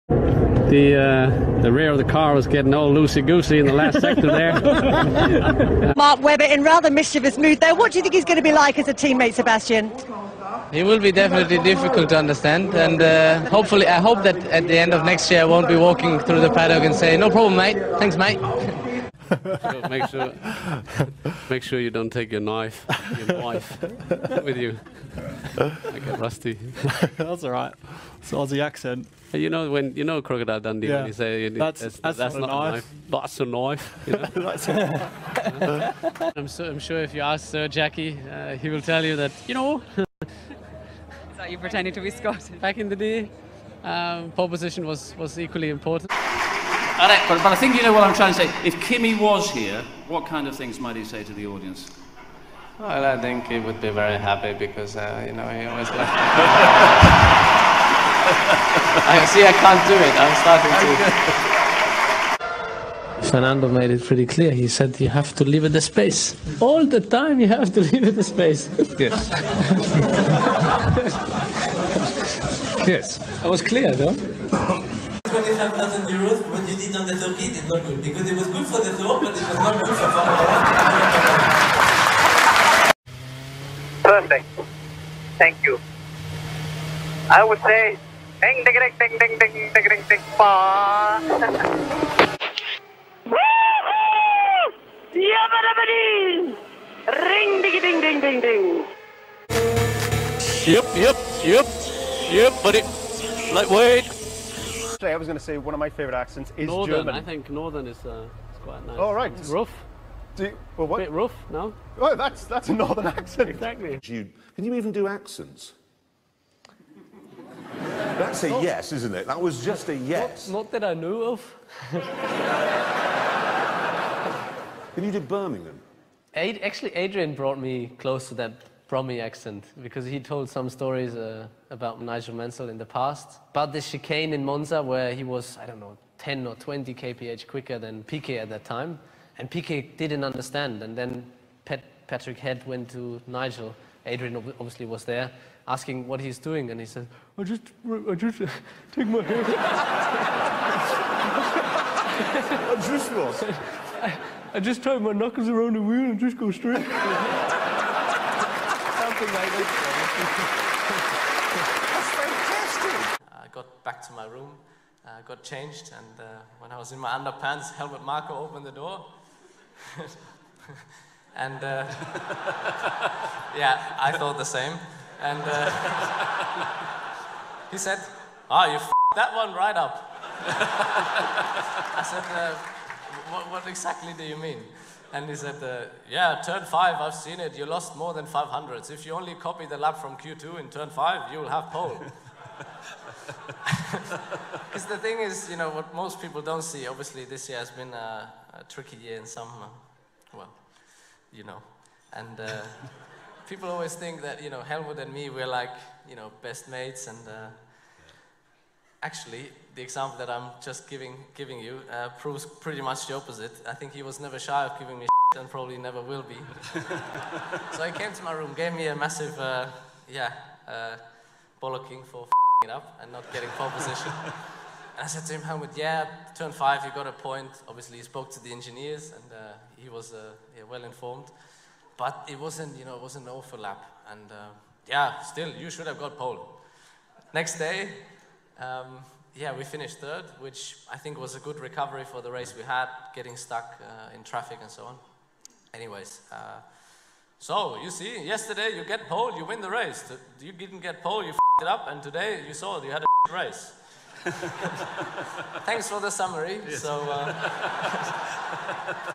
Sebastian Vettel: Accents and Impressions Compilation
A compilation of Formula 1 driver Sebastian Vettel showing off his arsenal of accents and impressions. From his Toro Rosso and Red Bull days to his Ferrari days. Hopefully, we can get a few more English impressions from him next year in Aston Martin.

Impressions and accents included:
0:00 - Texan accent
0:06 - Mark Webber Australian Accent
0:19 - Crocodile Dundee Australian Accent
0:36 - Sir Jackie Stewart Scottish Accent
0:46 - Kimi Räikkönen Impression
1:02 - Fernando Alonso Impression
1:15 - Jean Todt Impression
1:23 - Crazy Frog
1:39 - Ronnie Coleman Impression
1:45 - Northern English Accent
1:54 - Nigel Mansell Brummie Accent
2:43 - Helmut Marko Impression